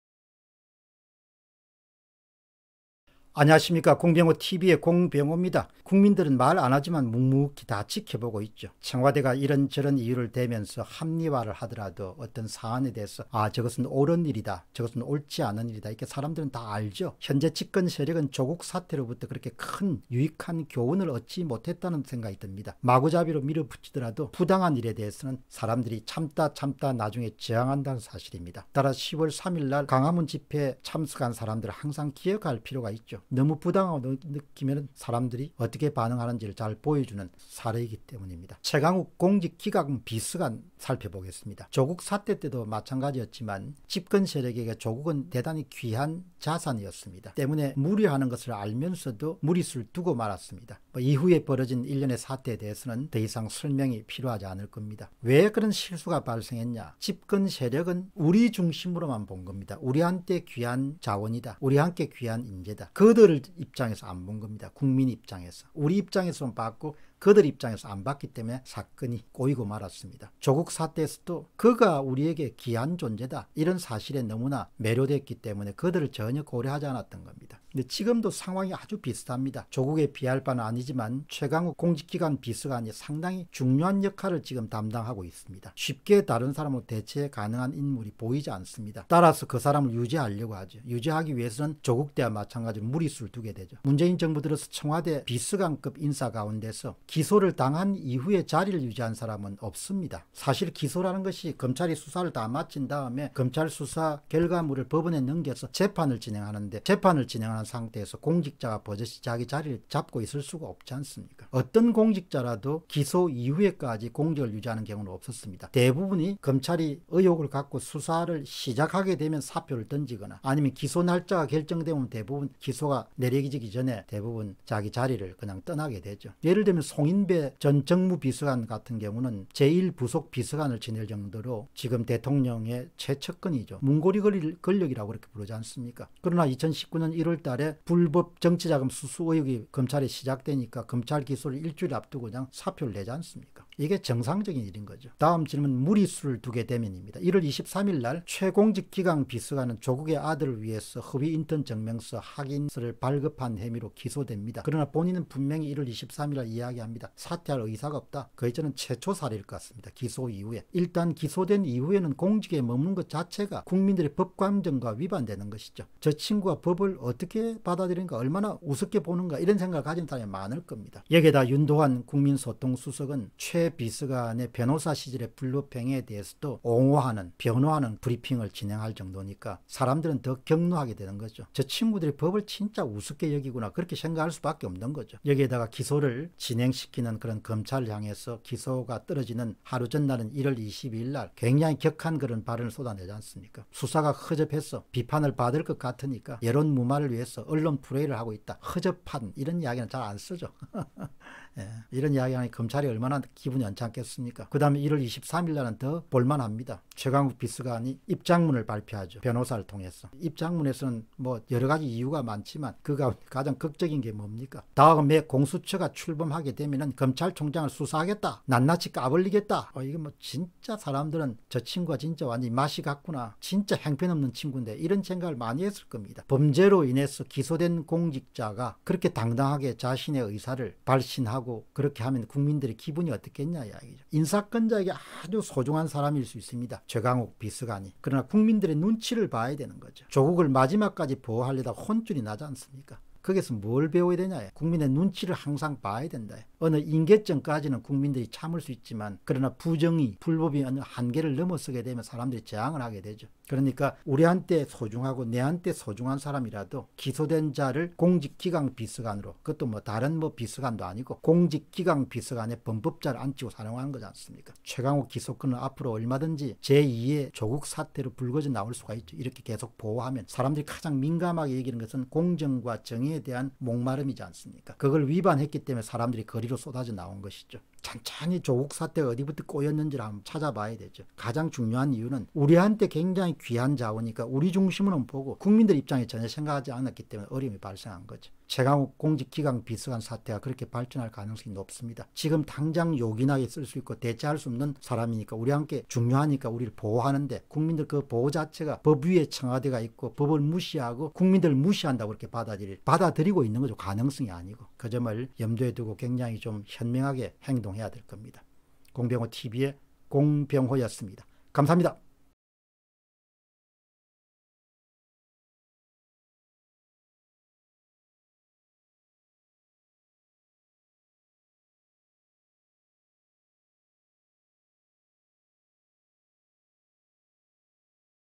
교만, 몰락의 지름길 / 최강욱 보호하기 [공병호TV]
교만, 몰락의 지름길
최강욱 보호하기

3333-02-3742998 카카오은행 (예금주 : 공병호) / (텔레뱅킹 : 090 & 은행코드 : 90)

1. 法 위의 靑… 압수수색 뭉개고, 비서관은 공수처로 검찰총장 협박, 박국희, 조선일보, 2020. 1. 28
2. 檢수사 건건이 반박한 靑대변인···최강욱 기소, 제2 조국사태 되나, 김수민, 중앙일보, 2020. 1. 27
3. 文정권서조차… 기소 후에도 현직 유지는 최강욱이 유일, 박국희, 조선일보, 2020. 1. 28